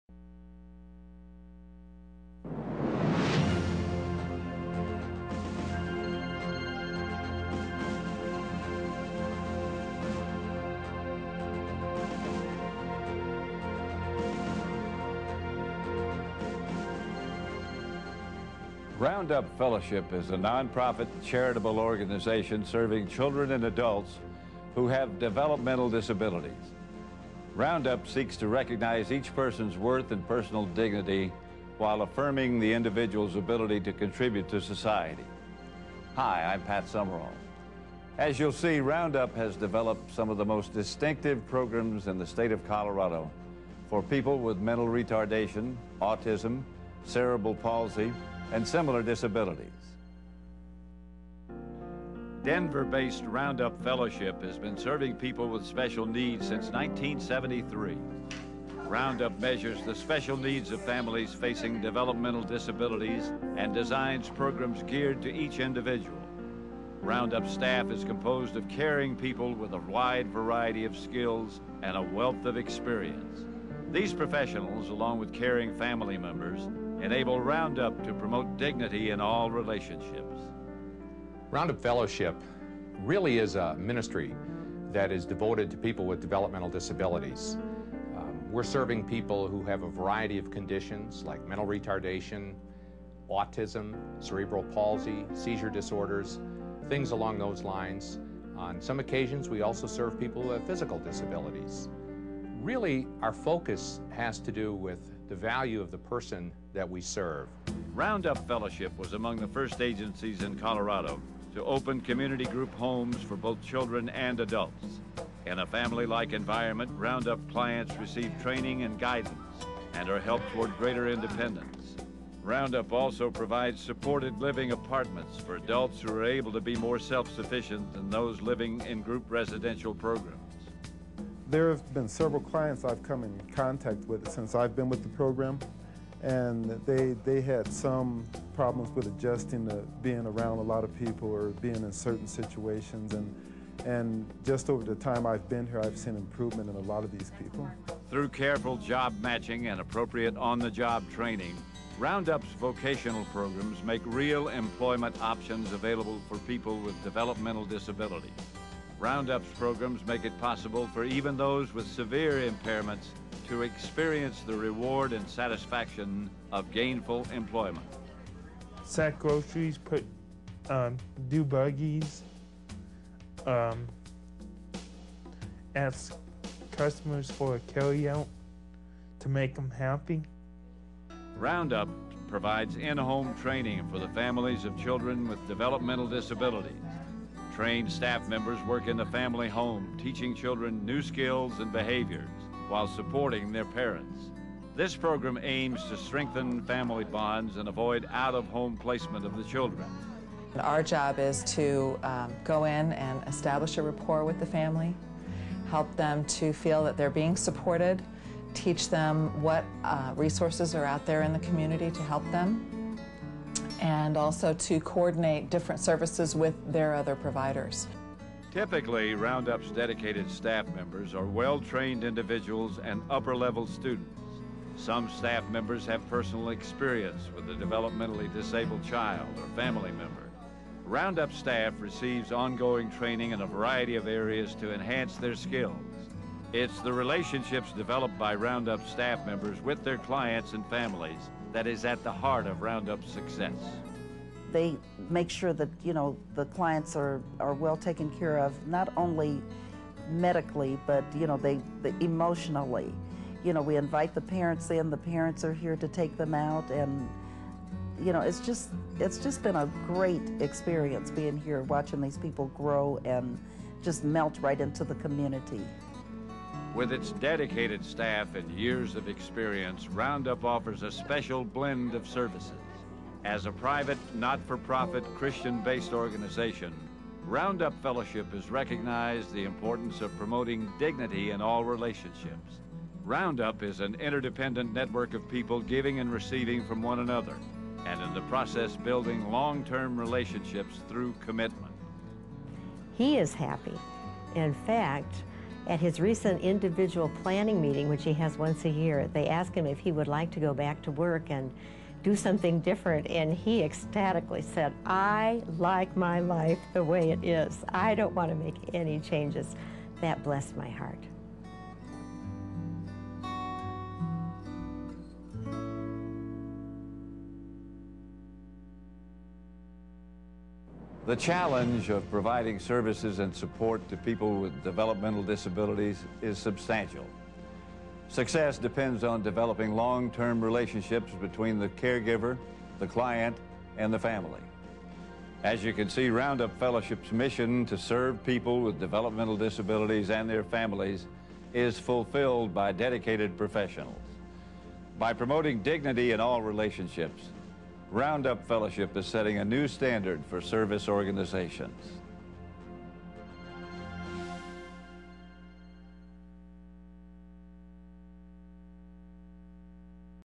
Roundup Fellowship 2002 Promotional Video
Roundup Fellowship's 2002 Promotional Video produced by Pat Summerall Productions.